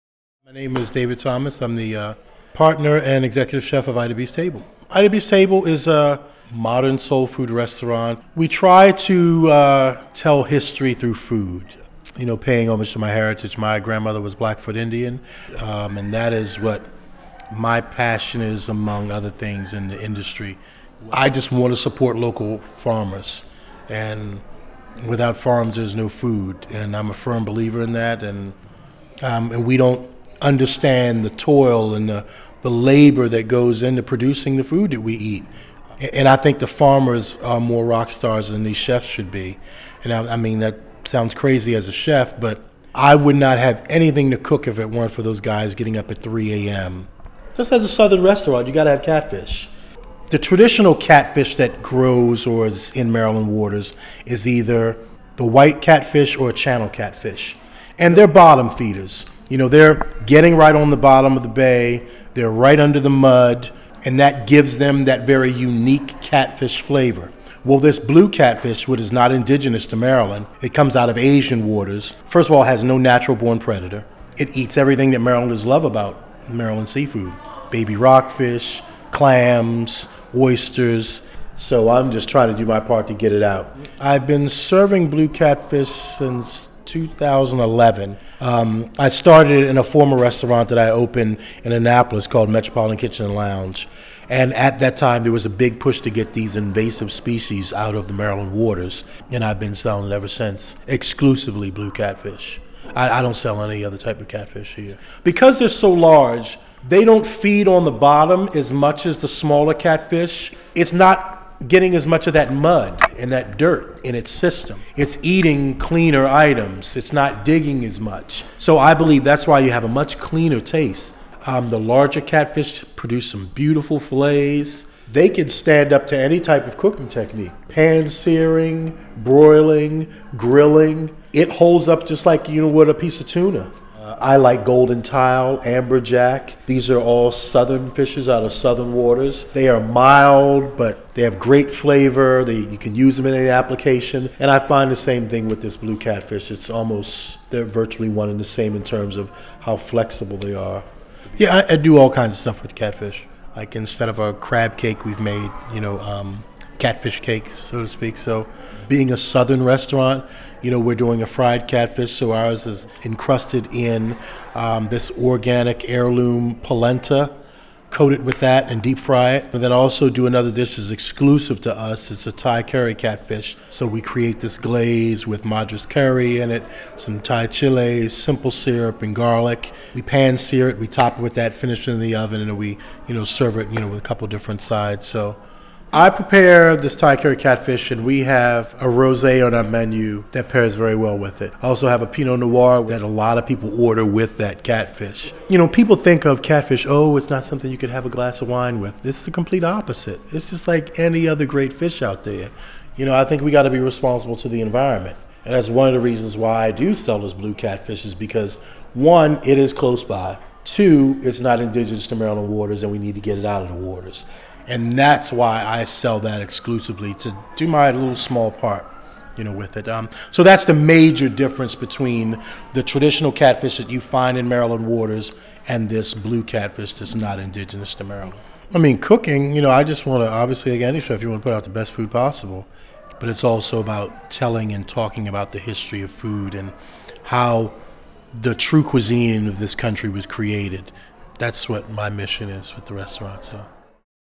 Ida B's Table
Chef David Thomas serves amazing dishes made with Maryland Blue Catfish at Ida B's Table in Baltimore